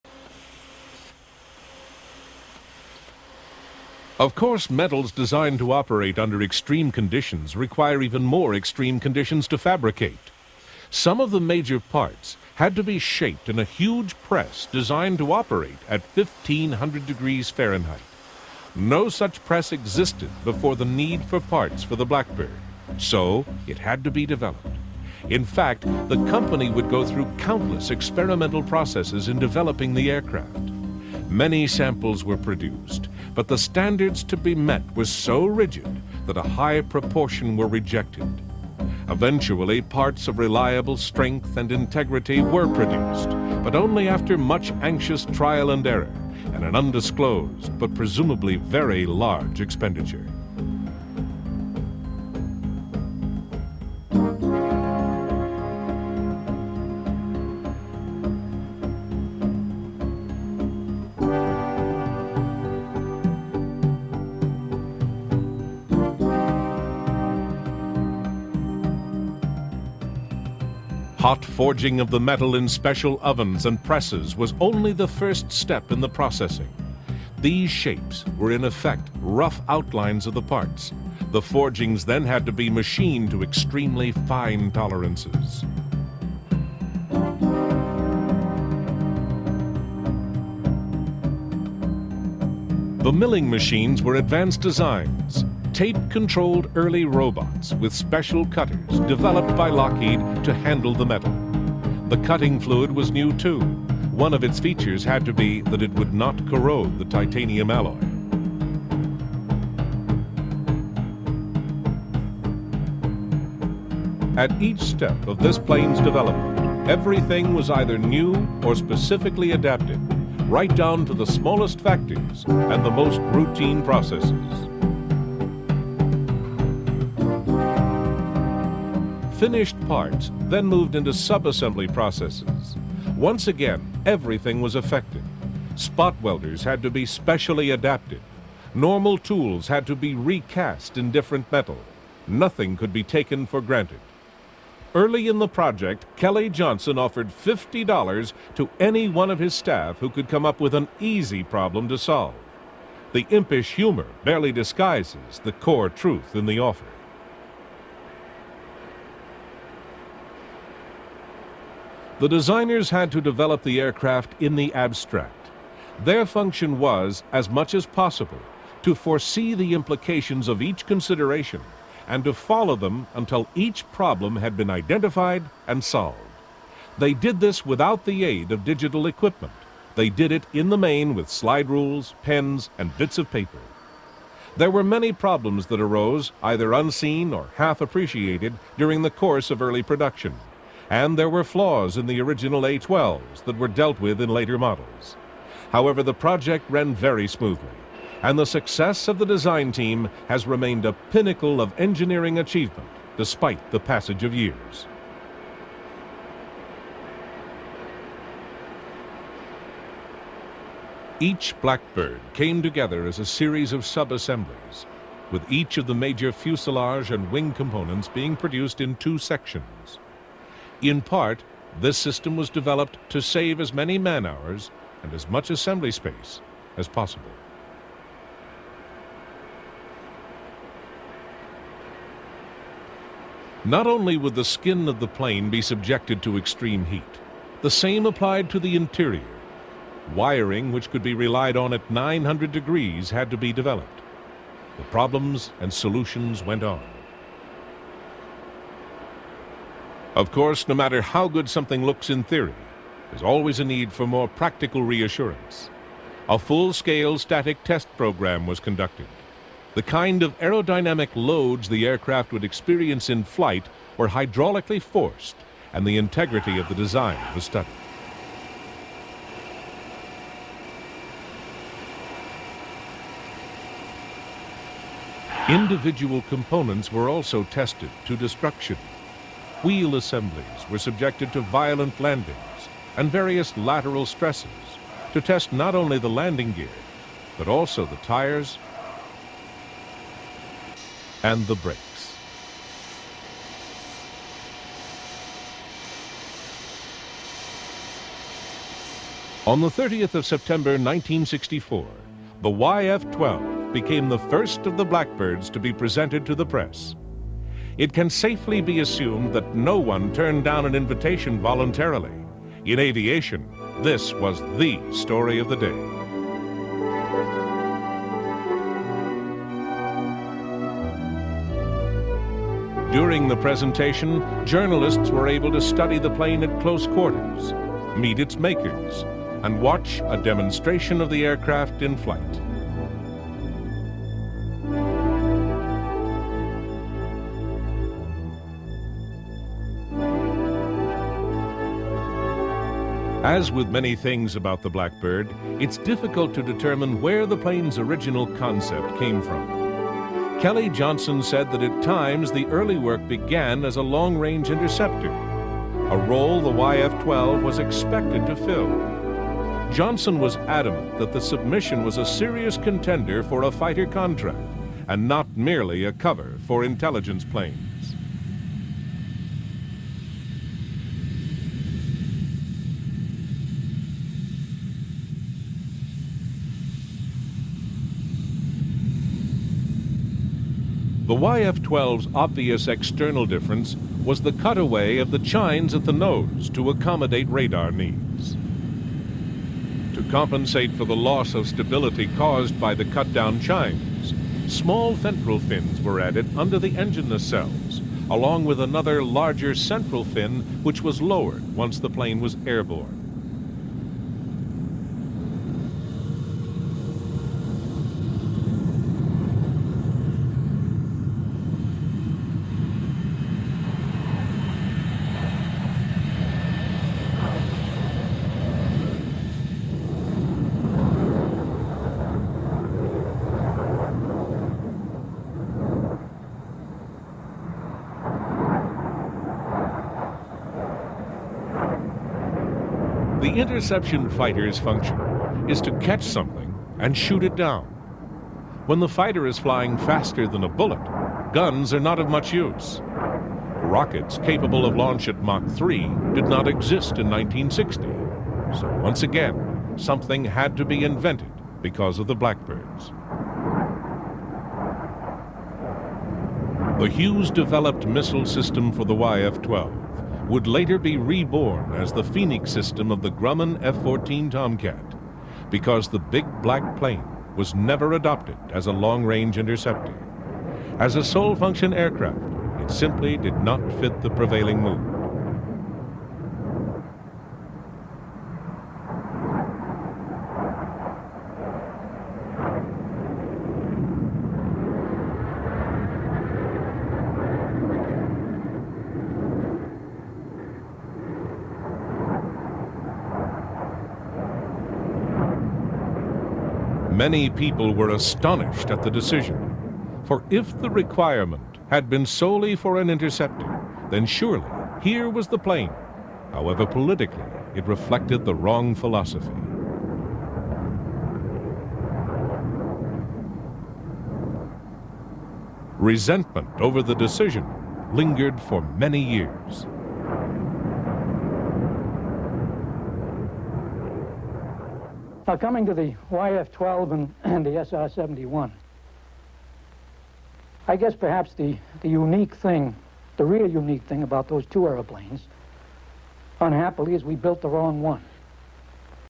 Sr-71 Blackbird Documentary Part 3
On the first of may 1965 an American plane designated YF-12a set 4 world records one for sustained altitude and three for speed including a mark of 2070 mph.